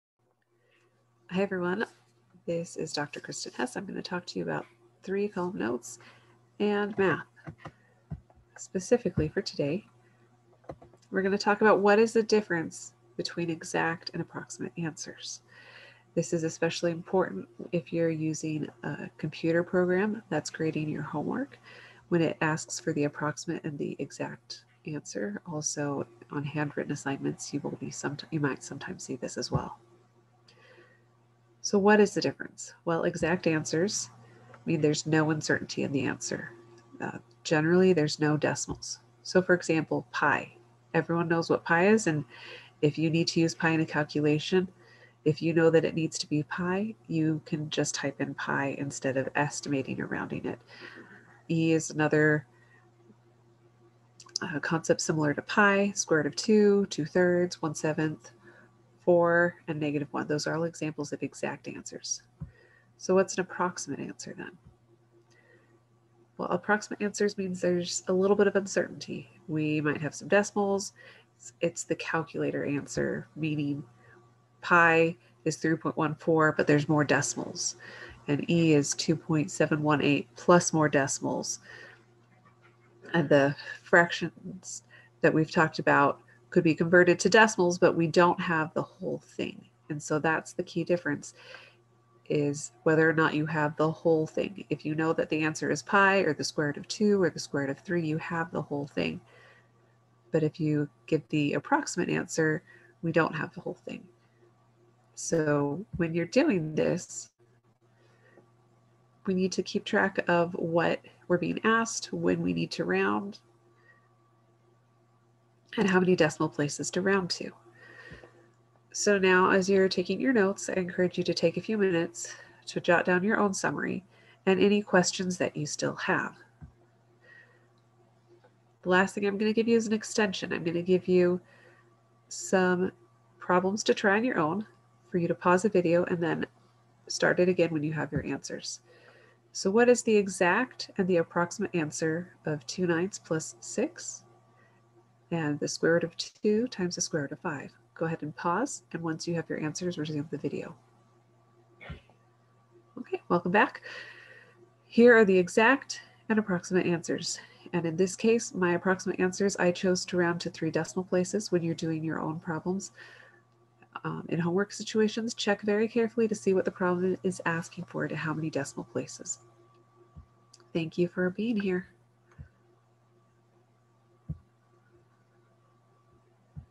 Exact vs. Approximate Answers in Math (3-column notes)
In this video, you will learn the difference between exact and approximate answers, why it matters, and where you will use it. The concepts will model using three column notes and give you opportunity to practice on your own as you watch the video.